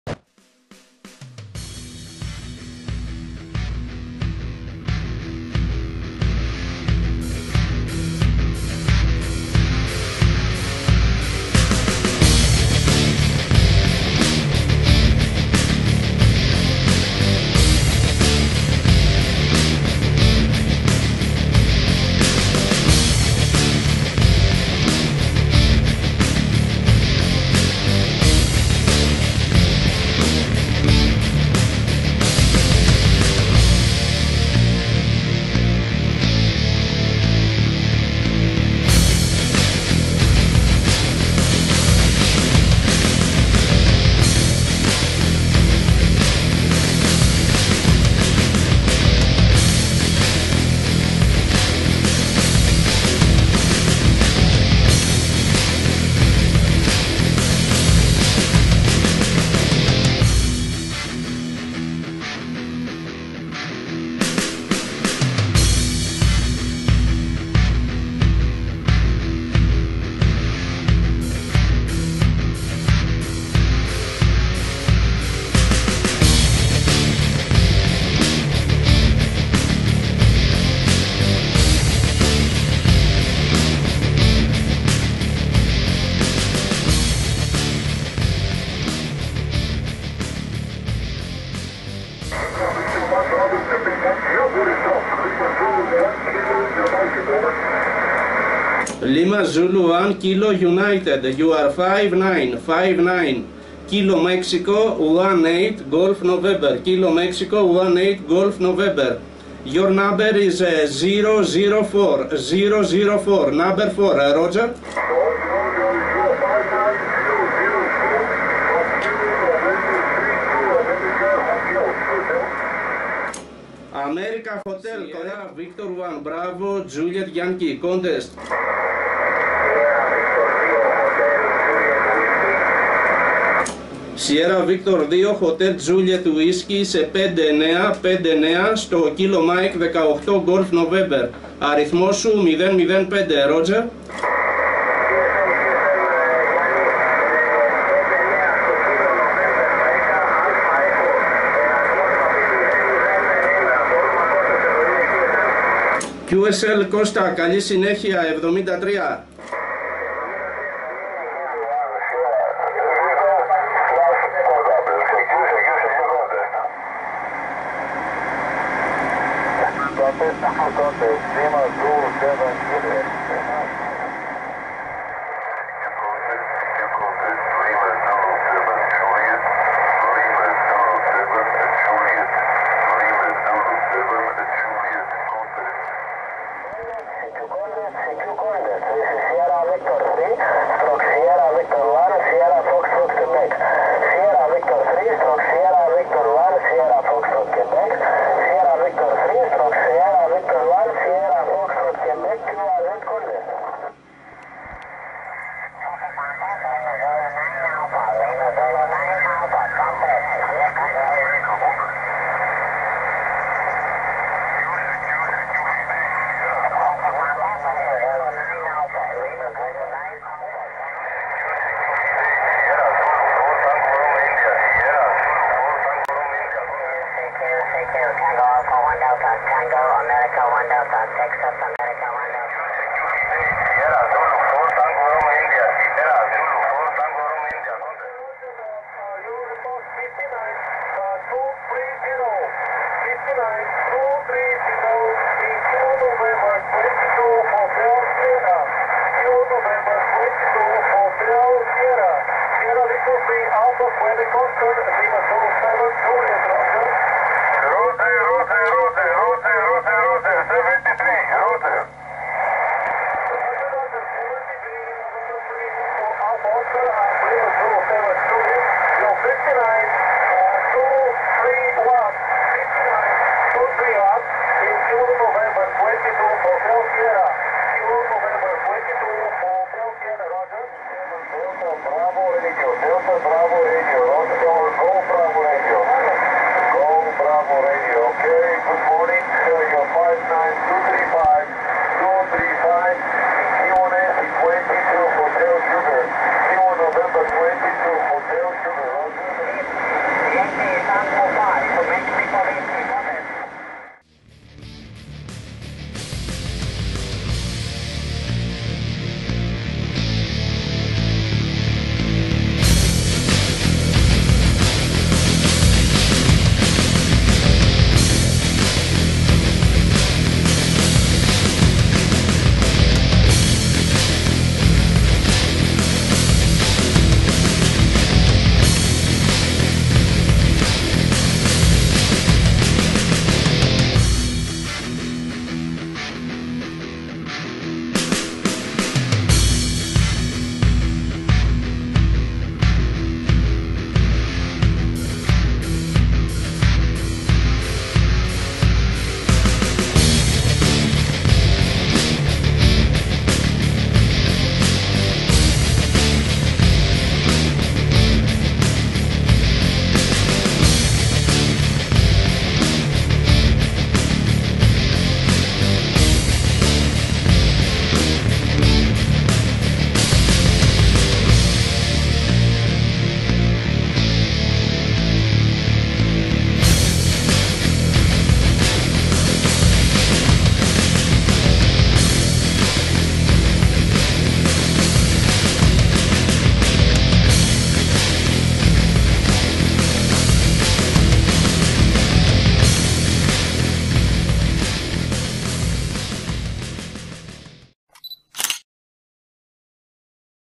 2019 VHF CTEST
2019 VHF CONTEST FOR IARU REGION 1 FROM 1800M ASL MT PARNASSOS. YANNIS-SV1BJY